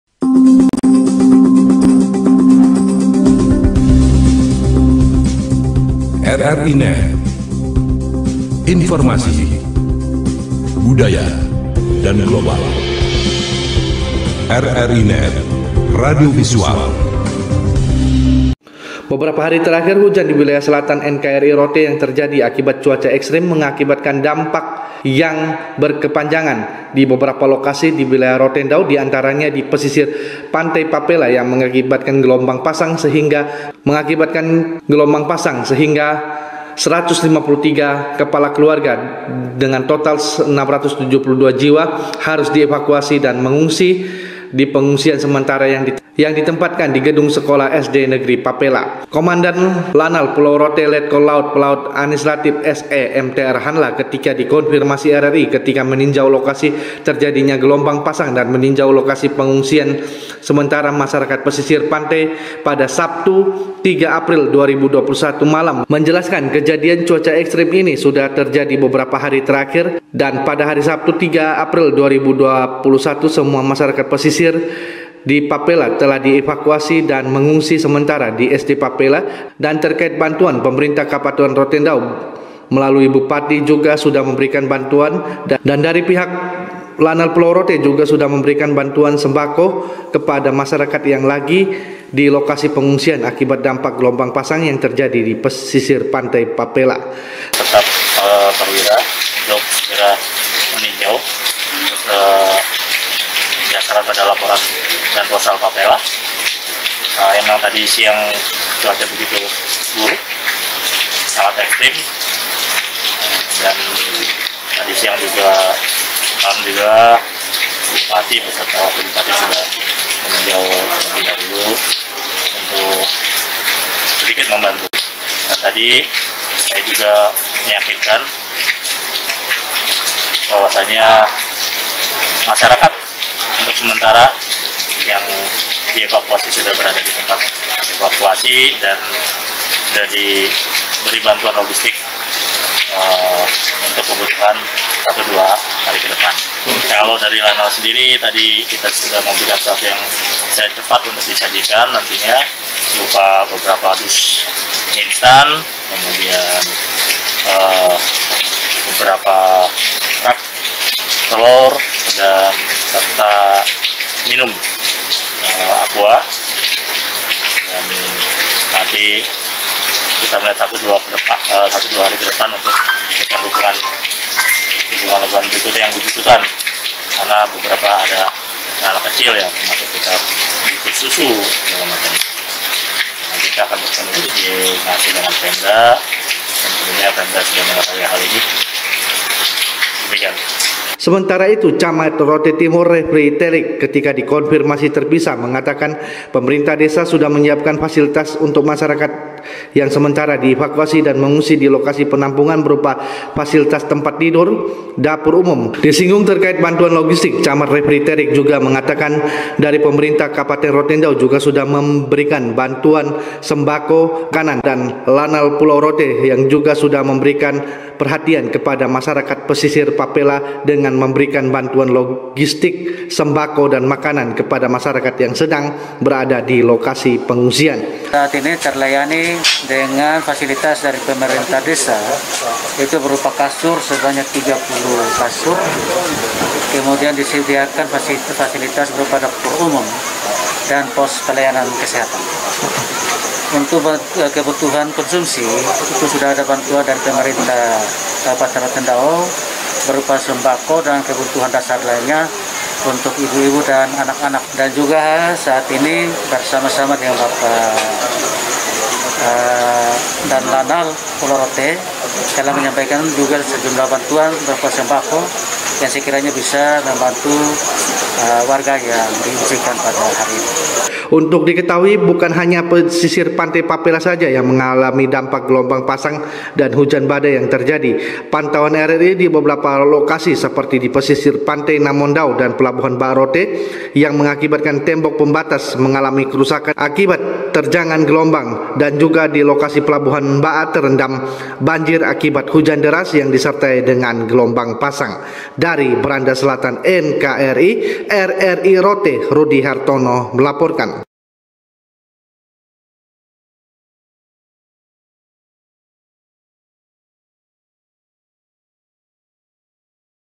PASCA TERJADINYA GELOMBANG PASANG DI PANTAI PAPELA KOMANDAN LANAL P ROTE TINJAU LOKASI KEJADIAN///
PASCA TERJADINYA GELOMBANG PASANG YANG TERJADI DI PESISIR PANTAI PAPELA KOMANDAN LANAL P ROTE MENINJAU MASYARAKAT YANG TERDAMPAK GELOMBANG PASANG///